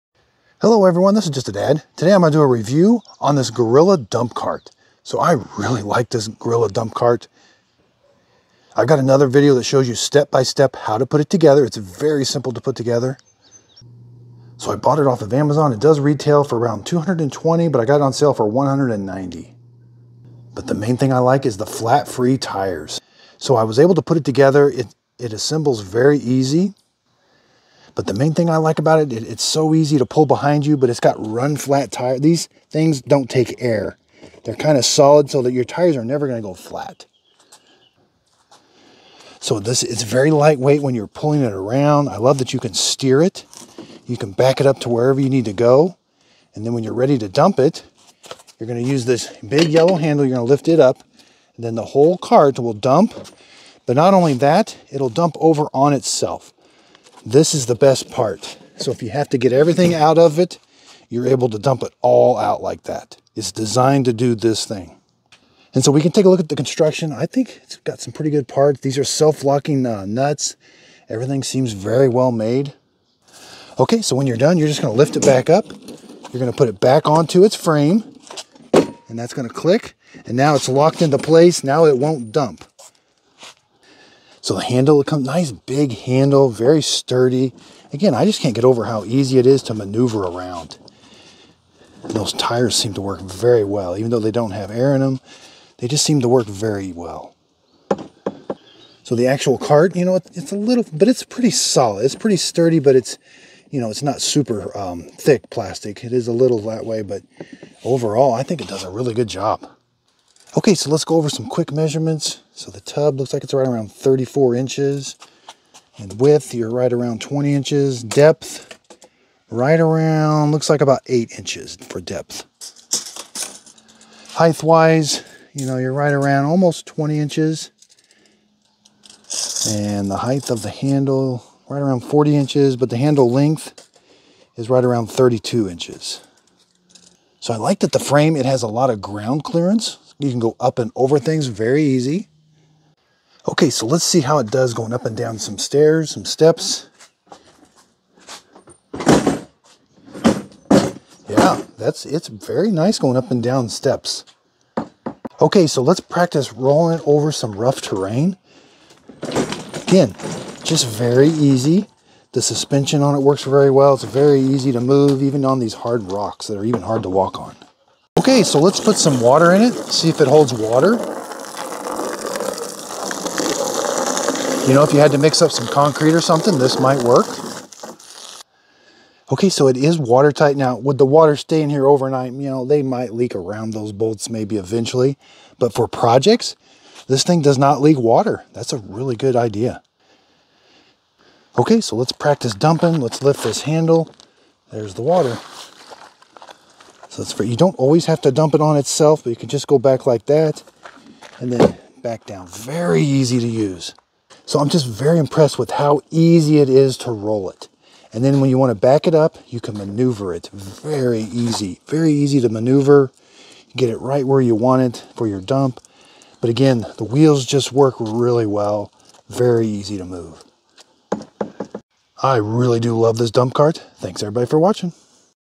REVIEW Gorilla Poly Garden Dump Cart with No-Flat Tires I LOVE IT!!!!
REVIEW Gorilla Carts 4GCG-NF 4 Cu. Ft. Poly Garden Dump Cart with No-Flat Tires How big is it and how does it roll over rocks and down hills.   How easy does it dump and is it water tight.